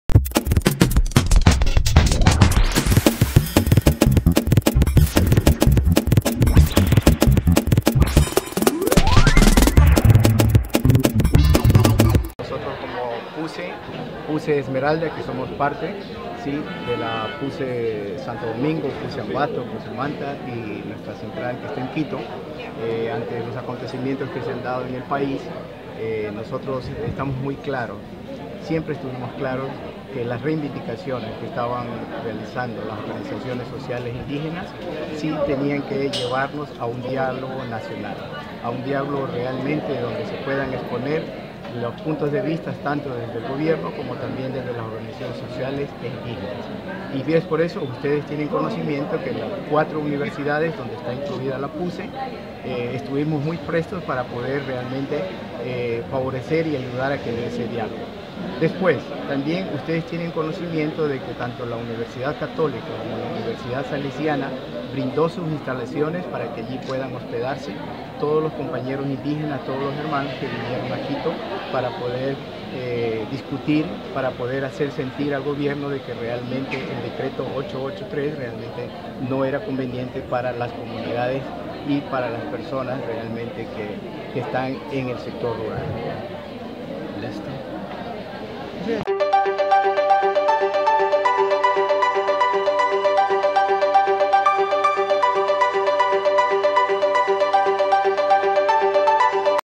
Lcd. Jacinto Fierro, DE LA PASTORAL SOCIAL PUCE ESMERALDAS.
Manifiesta que la universidad católica puso a disposición sus instalaciones para que se hospedaran los manifestantes indígenas.